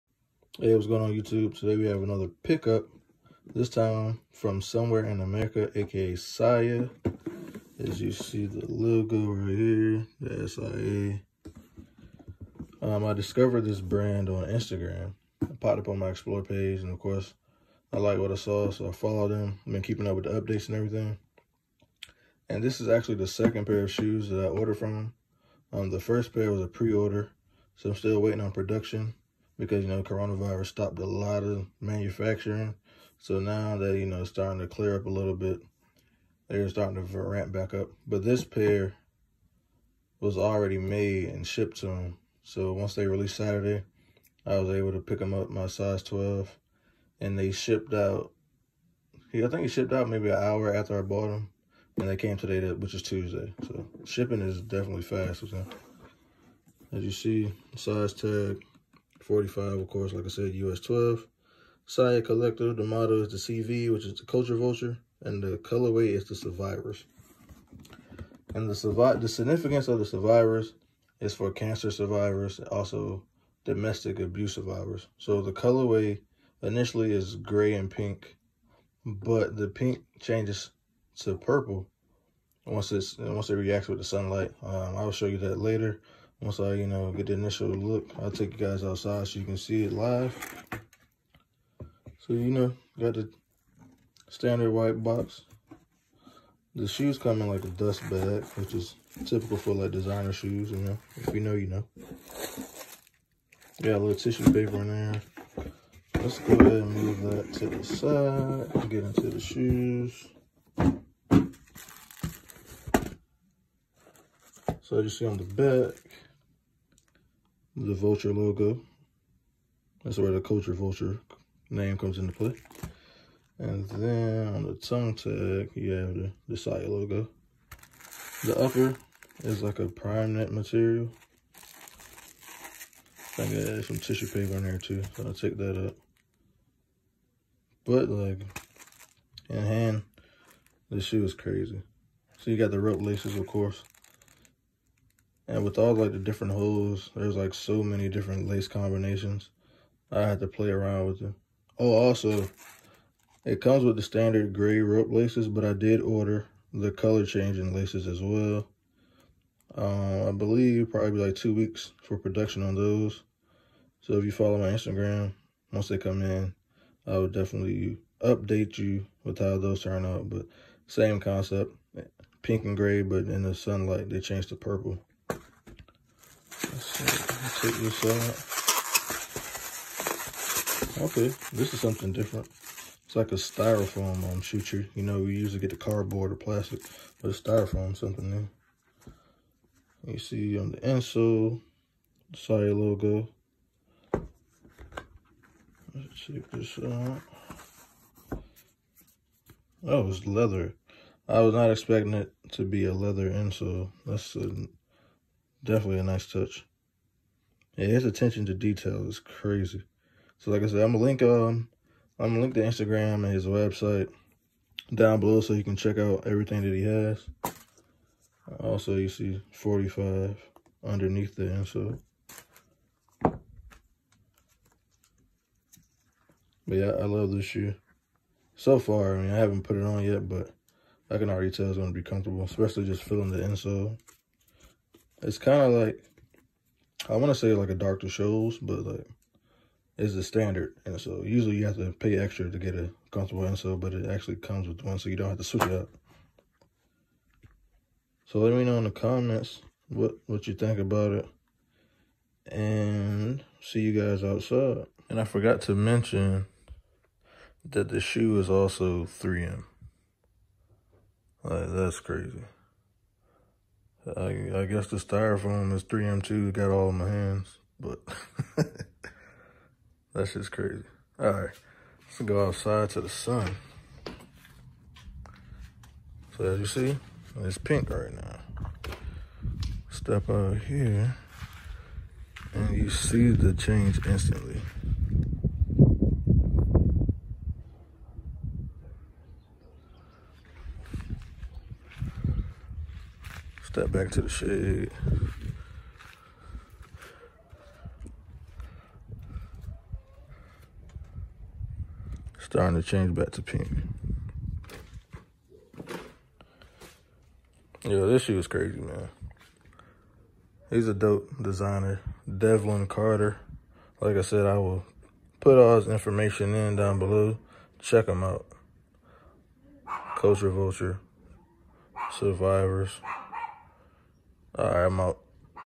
SIA Culture Vulture “Survivors” review [Color changing and reflective]
Sia collective culture vultures in the “Survivors colorway”
This colorway was created to honor breast cancer, lupus and domestic violence survivors

Personal